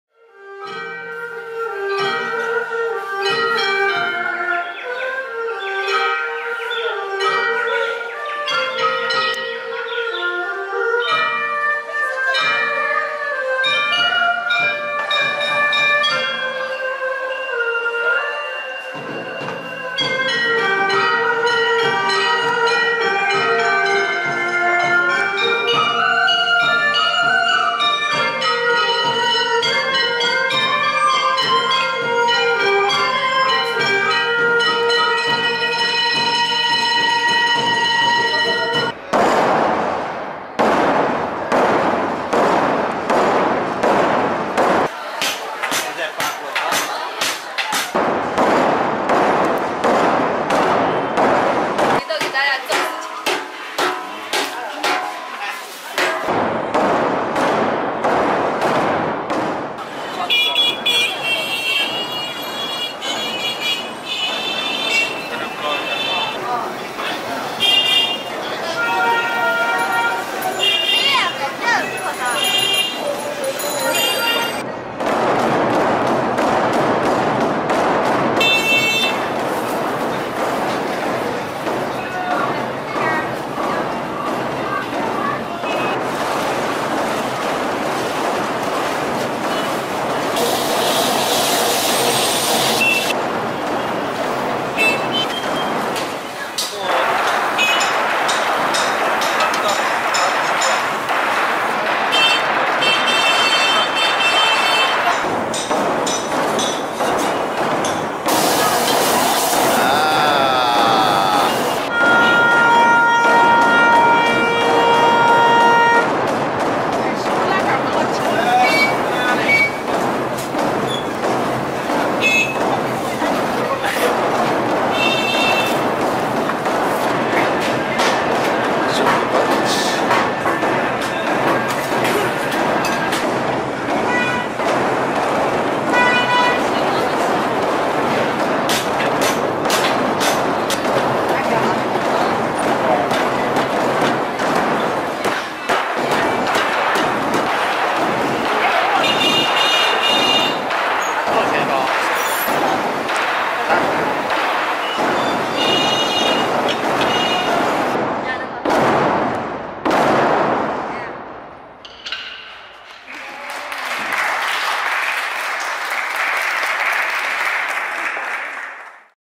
Shanghai 2016 (FastFood)
Suppenküchen in Shanghai's Altstadt. Aufgenommen von Andreas Frey 2016